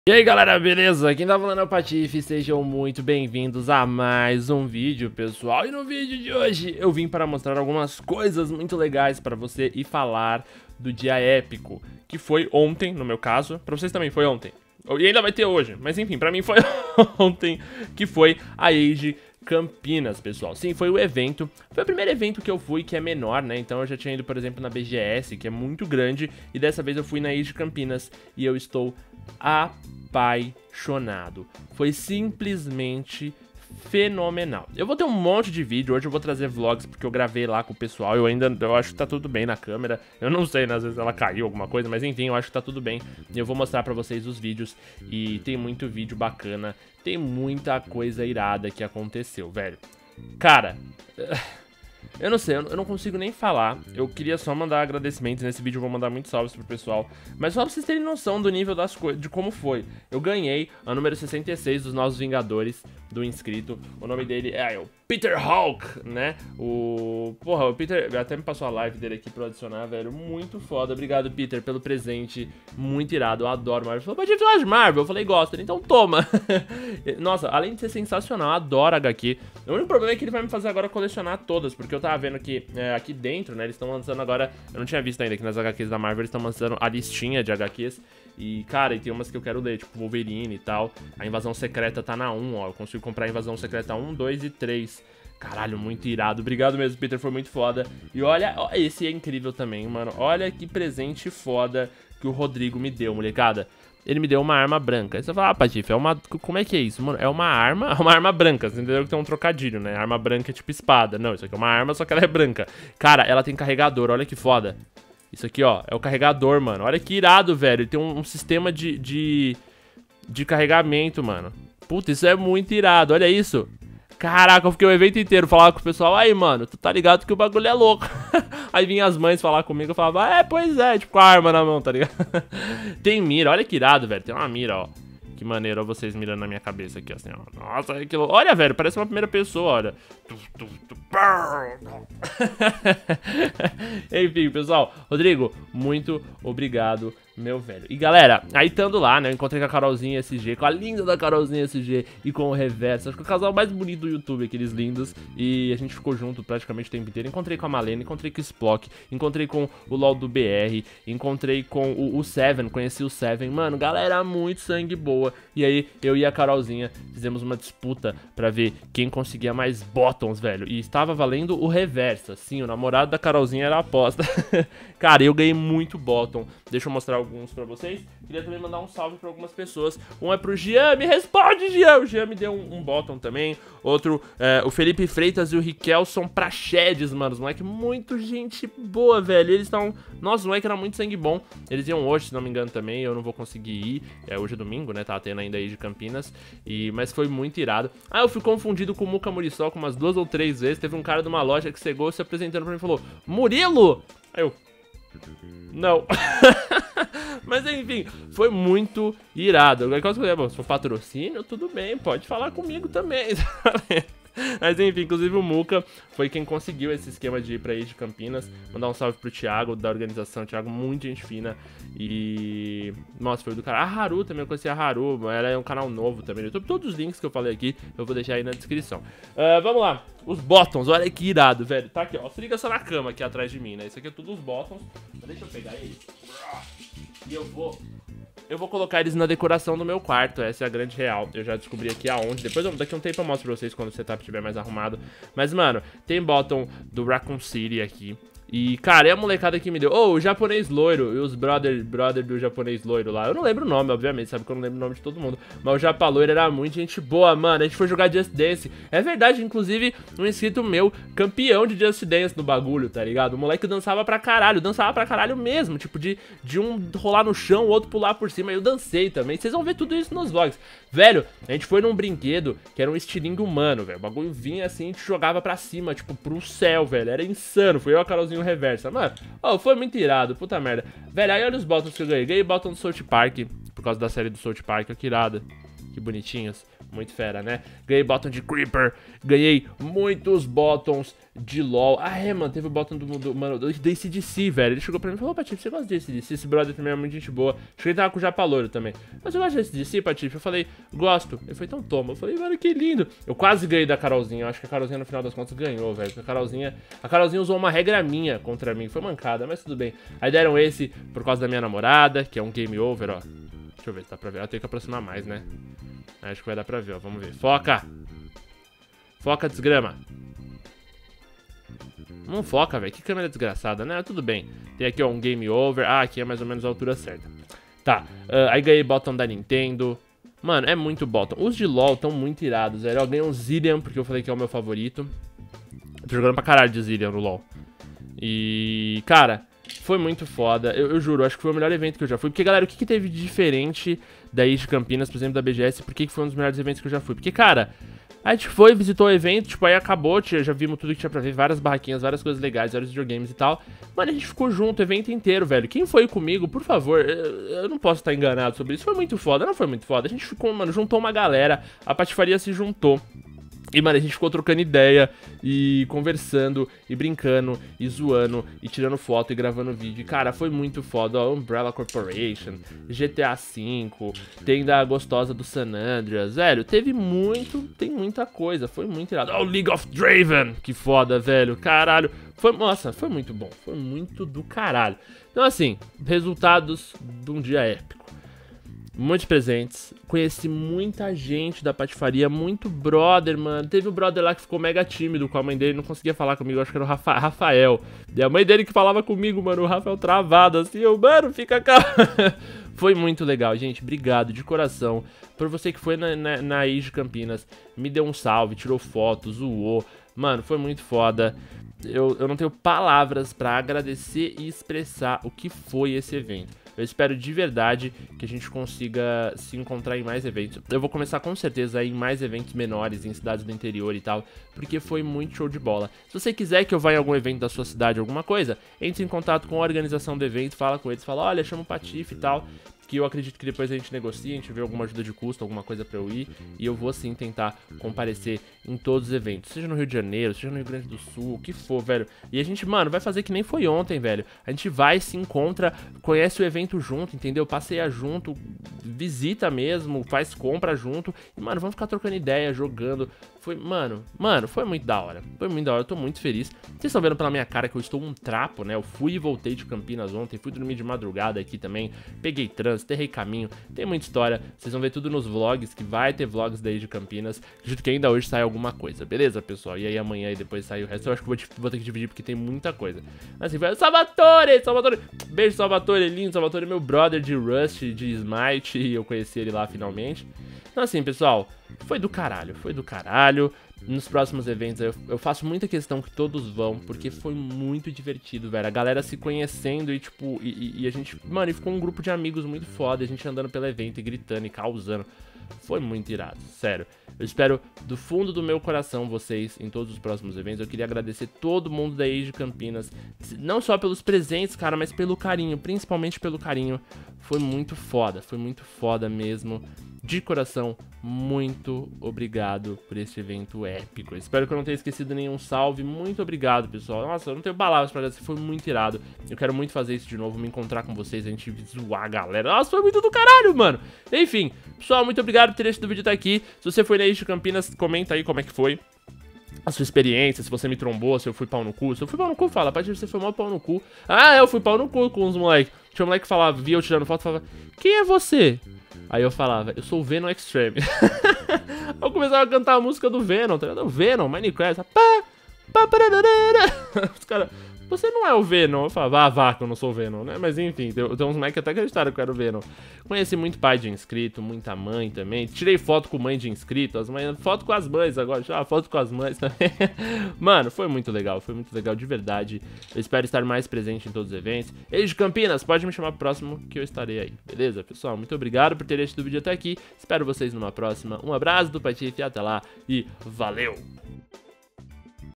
RESULTADOS DE UM DIA ÉPICO! OBRIGADO, VOCÊS ME FIZERAM MUITO FELIZ! =D
Nesse vídeo vou conversar um pouco com vocês sobre minha épica viagem pra Campinas =D

Skype: gamer.patife
Valeeu!